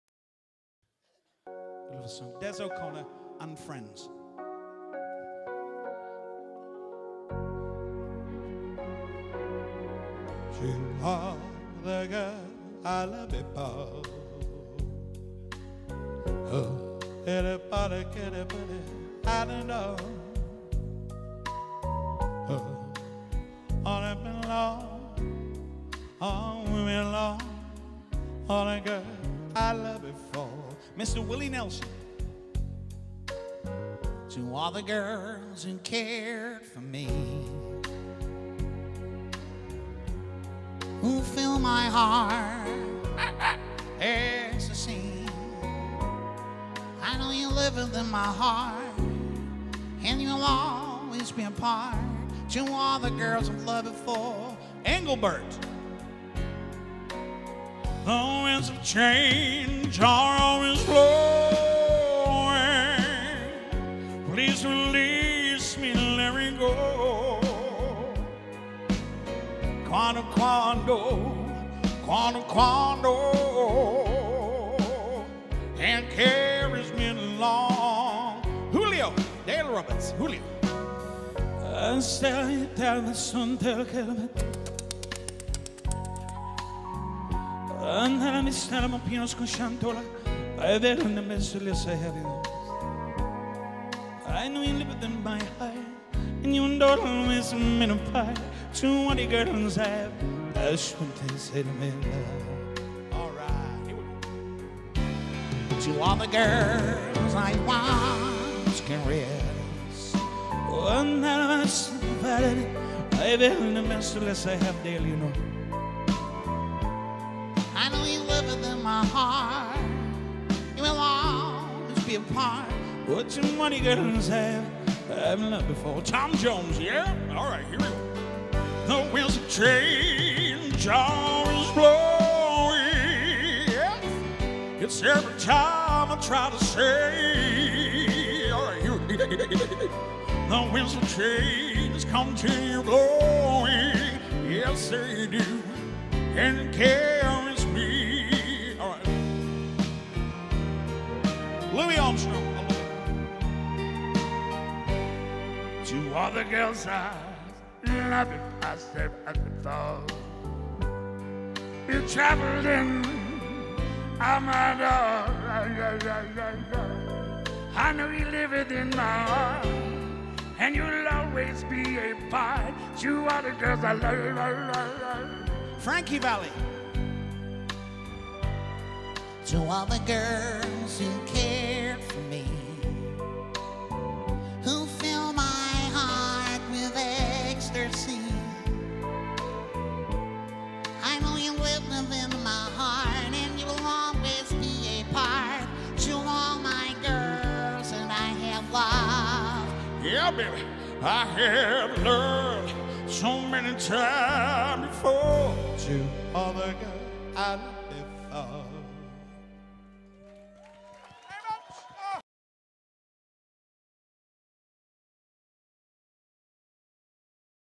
Marc Bolton - The Entertainer Awards 2016
Marc Bolton won the best Comedy/Vocalist award in the Entertainer magazine 2015/16...to book Marc please call Dukeries Entertainments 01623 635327 or email